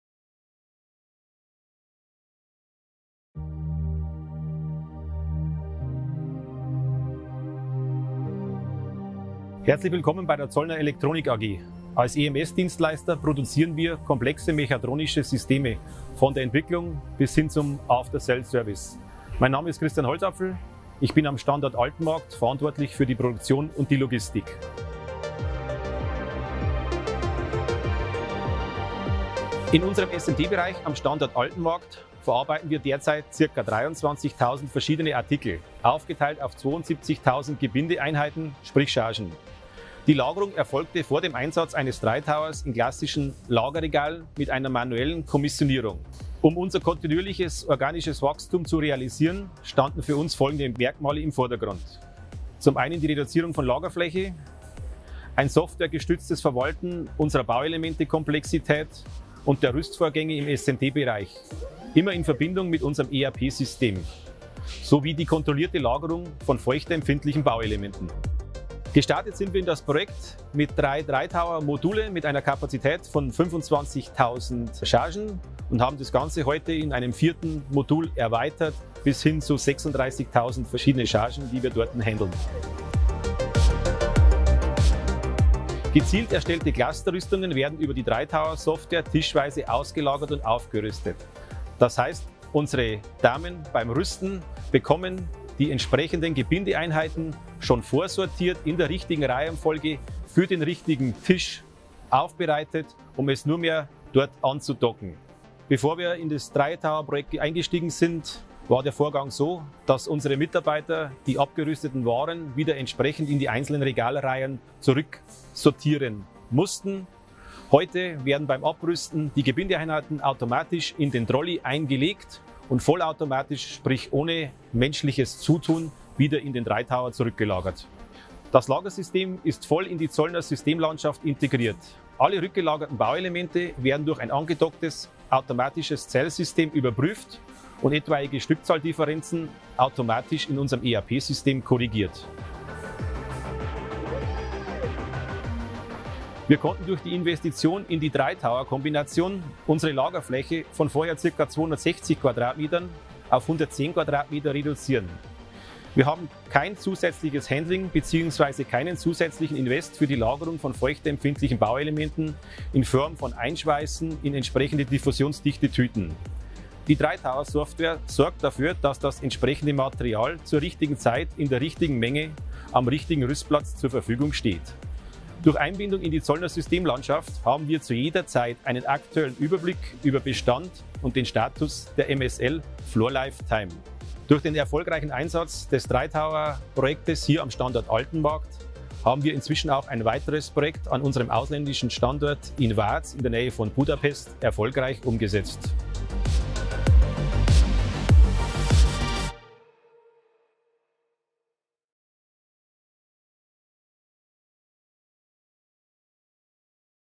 Automatisiertes Lagersystem bei der Firma Zollner – Totech Dry Tower
Im SMD Bereich am Zollner Standort Altenmarkt werden 23.000 verschiedene Artikel, aufgeteilt auf 72.000 Gebindeeinheiten, gehändelt.
Die Lagerung und die Kommissionierung erfolgte vor dem Einsatz des Dry Towers manuell.

Aufgrund vom stetigen organischen Wachstum stand Zollner vor der Herausforderung, die Prozesse im SMD-Bereich zu optimieren.
Unter Anderem sollte die Lagerfläche reduziert und die Lagerung der feuchtempfindlichen Komponenten sichergestellt werden.

Im ersten Schritt wurden drei Dry Tower Quattro Module mit einer Kapazität von 25.000 Chargen eingesetzt und später um ein viertes Modul erweitert, um bis zu 36.000 verschiedene Chargen händeln zu können.
Durch die Investition in die Dry Tower Kombination konnte nicht nur die Lagerfläche reduziert, sondern außerdem viele Prozesse optimiert werden. Die Dry Tower Software sorgt dafür, dass das entsprechende Material zur richtigen Zeit in der richtigen Menge, am richtigen Rüstplatz zur Verfügung steht und durch die Einbindung in die ERP-Software behält der Kunde zu jeder Zeit einen Überblick über den Bestand und den Status der MSL Floor Lifetime.
Durch den erfolgreichen Einsatz des Dry Tower Projektes an einem Standort in Deutschland hat sich Zollner zudem für die Installation eines weiteren Dry Towers in Ungarn entschieden.

Schauen Sie sich das Video an, um weitere Informationen zu dem Projekt zu bekommen.